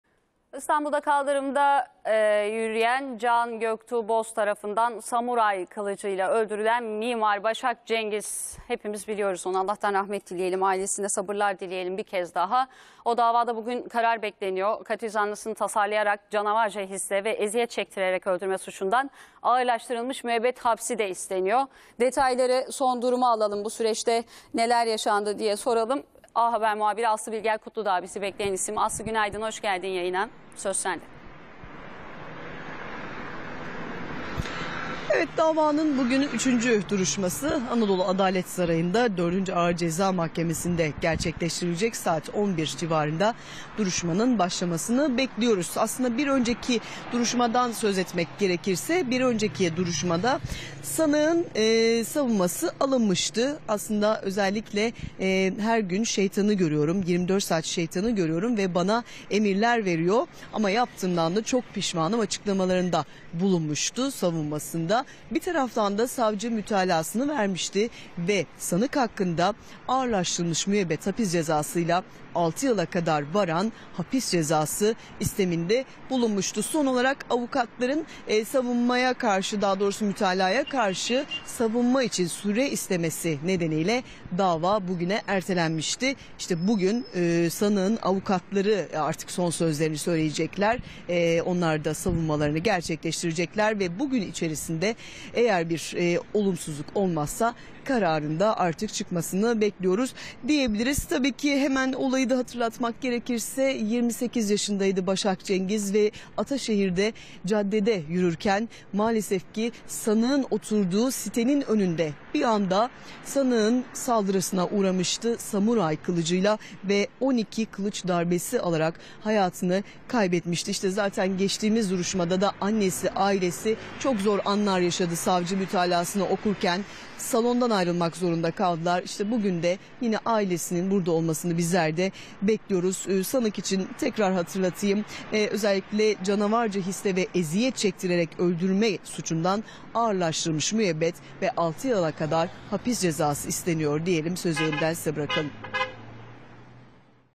Mimar Başak Cengiz'i katletmişti! Can Göktuğ Boz'un yargılanmasında kritik gün! | A Haber
İstanbul'da kaldırımda yürürken Can Göktuğ Boz tarafından samuray kılıcıyla öldürülen mimar Başak Cengiz davasında bugün karar bekleniyor. Katil zanlısının "Tasarlayarak canavarca hisle ve eziyet çektirerek öldürme" suçundan ağırlaştırılmış müebbet hapsi isteniyor. Ayrıntıları A haber muhabiri Aslı Bilger Kutuldağ aktardı.